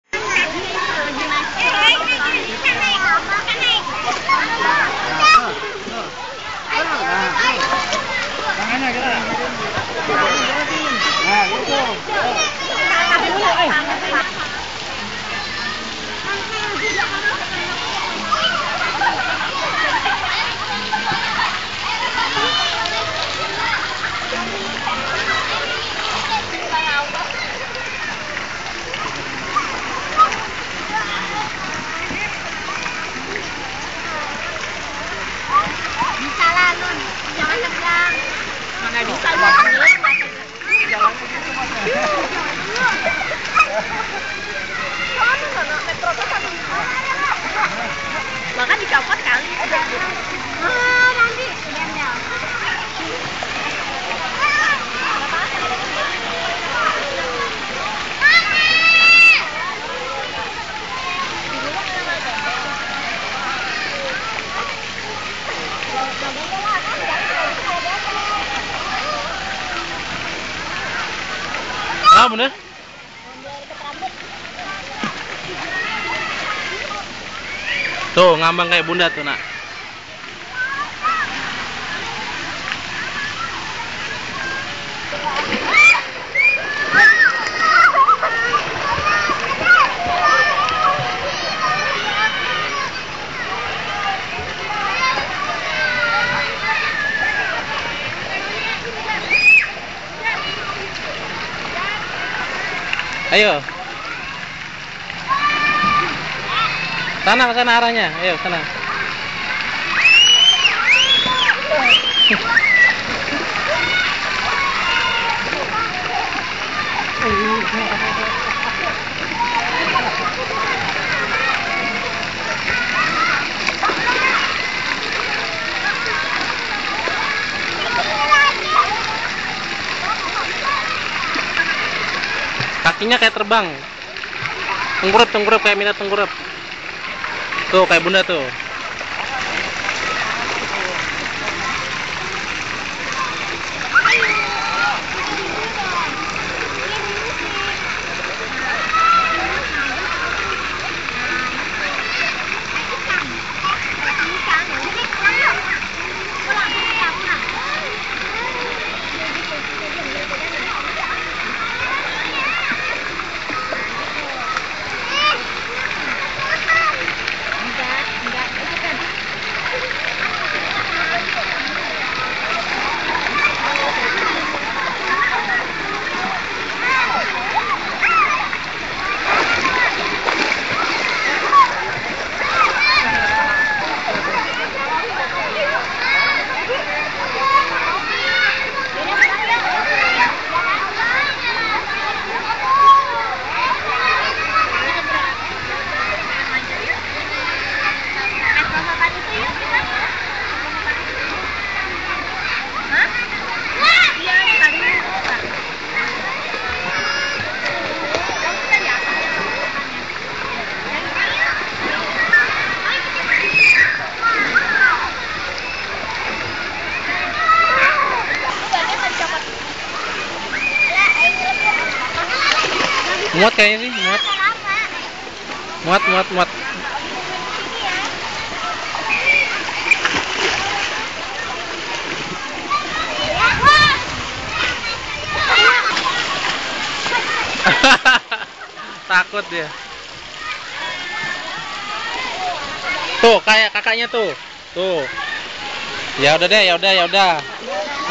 MITA & WATERBOOM LIPPO CIKARANG (PART 2/11)
Sabtu, 4 Juni 2011 (jam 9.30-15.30)

Sayangnya, video ketika meluncur dari tower "PITA DEBAR-DEBAR" tak bisa direkam, karena butuh "peralatan khusus".

2 hari kemudian, mulai Senin s/d Rabu, Mita terserang sakit telinga, hidung & tenggorokan serta demam tinggi (40 C), batuk-batuk & pilek. Setelah mendapatkan pengobatan dari Tante dr. Vera Tantiyono , SpA pada hari ke-4 suhu tubuh normal kembali sampai sekarang.

Karena sempat sakit, Eyangnya membatalkan tiket nonton "Sleeping Beauty" di GKJ supaya Mita lebih banyak istirahat di akhir minggu ini. Tapi bukannya istirahat malah belanja susu ke Tip Top, Rawamangun & jalan-jalan ke Mall Kelapa Gading ???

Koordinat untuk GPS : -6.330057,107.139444